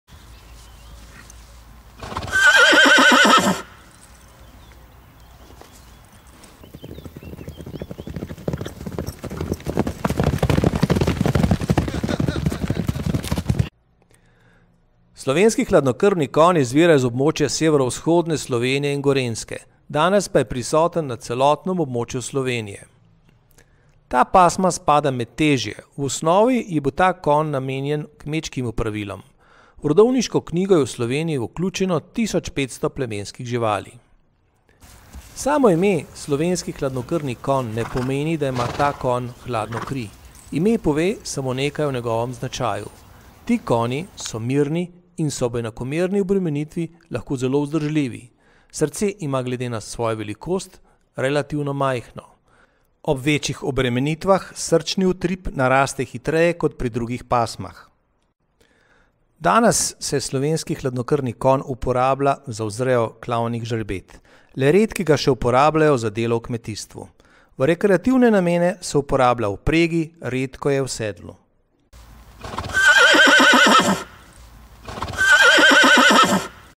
Slovenske avtohtone pasme: SLOVENSKI HLADNOKRVNI KONJ
Slovenski hladnokrvni konj izvira z območja severovzhodne Slovenije in Gorenjske in je razširjen po celotnem območju Slovenije. V izvorno rodovniško knjigo je bilo leta 2019 vključenih okoli 1500 plemenskih živali. Zaradi relativno visoke stopnje sorodnosti med konji v populaciji, slovenskega hladnokrvnega konja uvrščamo med kritično ogrožene pasme. Večinoma je pasma namenjena vzreji klavnih žrebet ter za prirejo in predelavo kobiljega mleka, nekateri rejci pa živali še vedno uporabljajo za pomoč v kmetijstvu in v manjši meri pri spravilu lesa iz gozda. Uporabljajo jih tudi kot rekreativne konje v vpregi, redkeje pa pod sedlom.